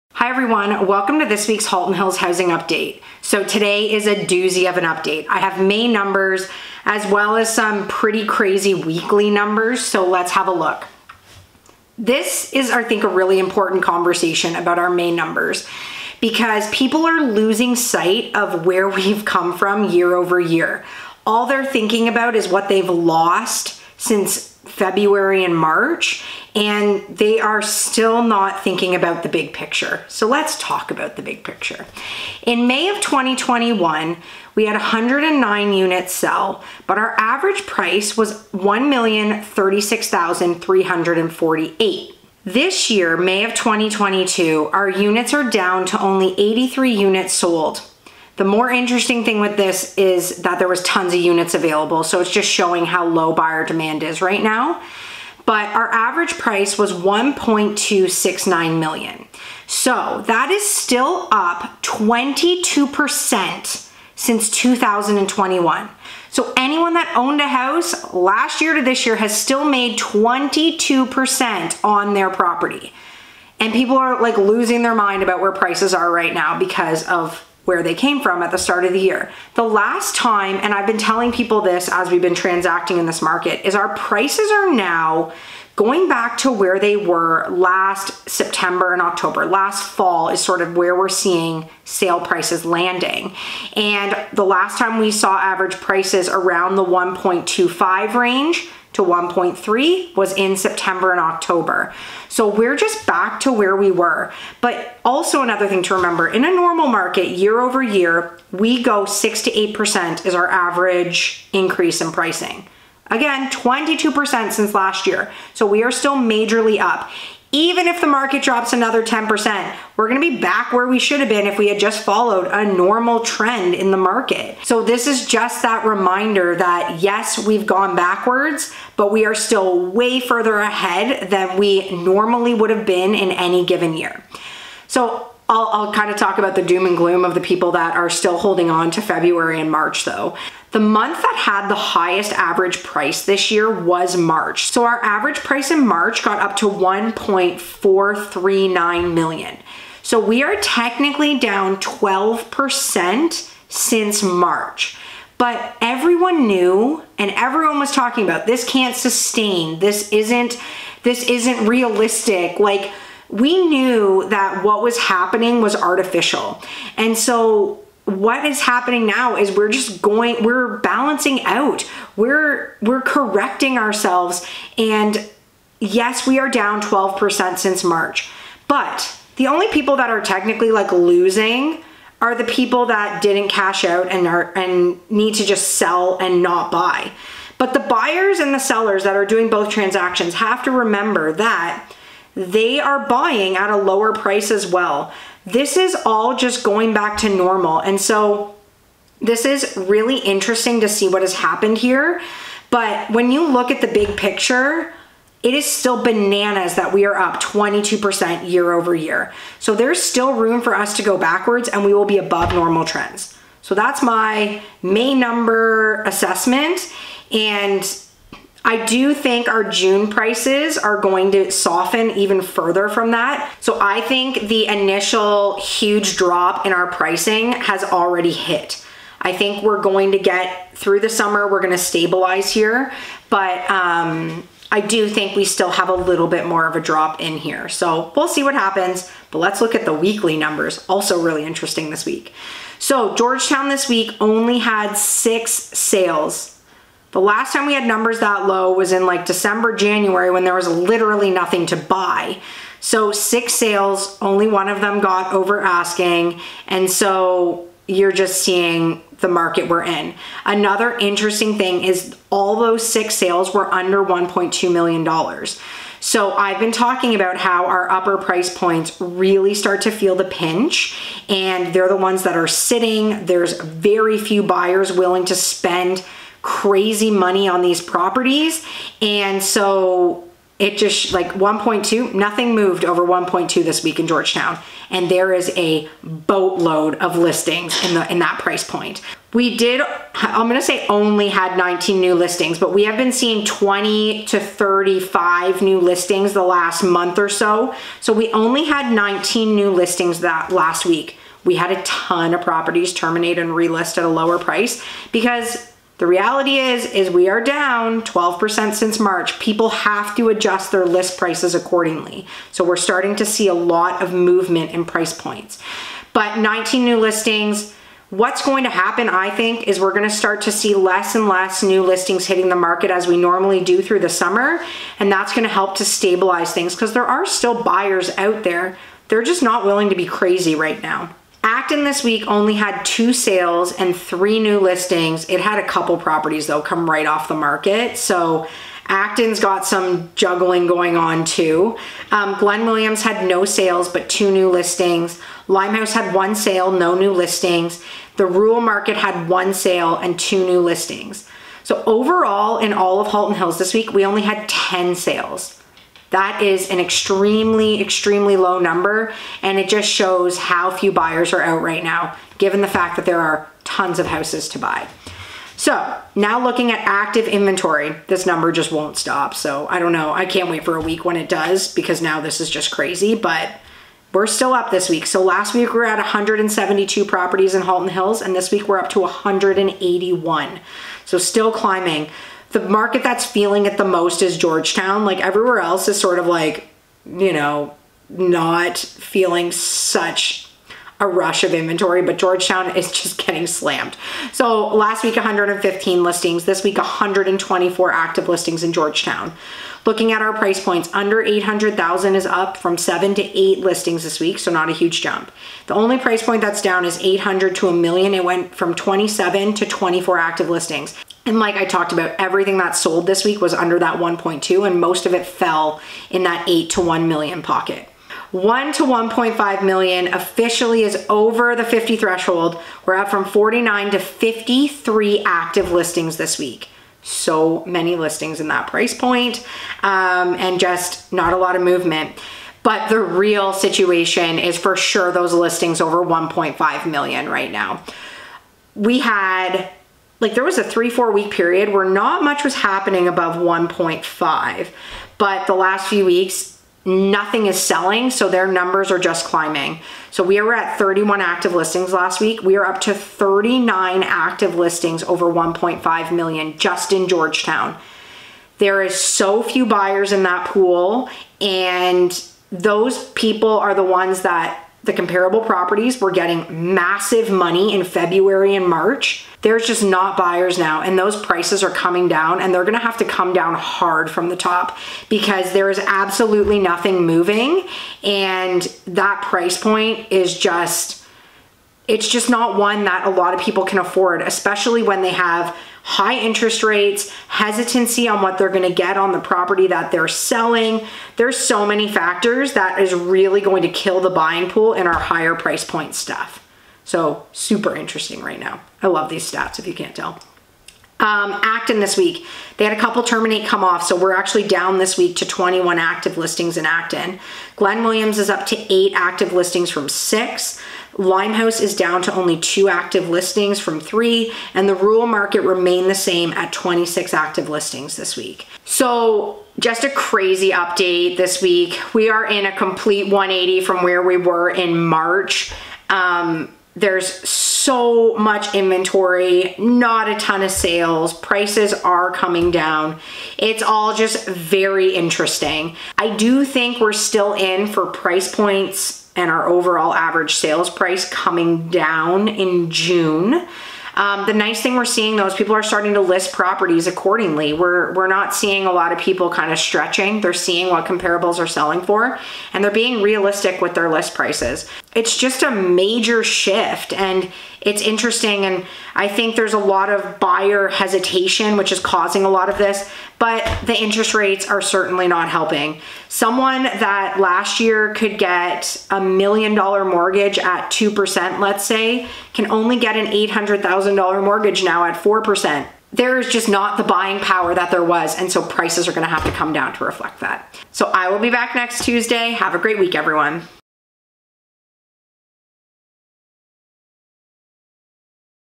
Halton Hills Housing Update - Ep. 122
May numbers are in‼️ and this update is so good and so important. People are losing their minds on prices dropping but I’m here to remind everyone prices are still up 22% year over year! Yes we are down 12% since March but I talk about both of these things in depth in todays update! It’s a goodie!

After that we review the weekly numbers. We only had 10 sales in all of Halton Hills this week. The stats are not good for sellers as inventory continues to climb and the buyer count seems to be shrinking.

I do anticipate we are in for a bit more of a hit to our average prices but I really believe the bulk of the drop has already been felt. We will eventually stabilize but for now, it appears we aren’t close to that yet.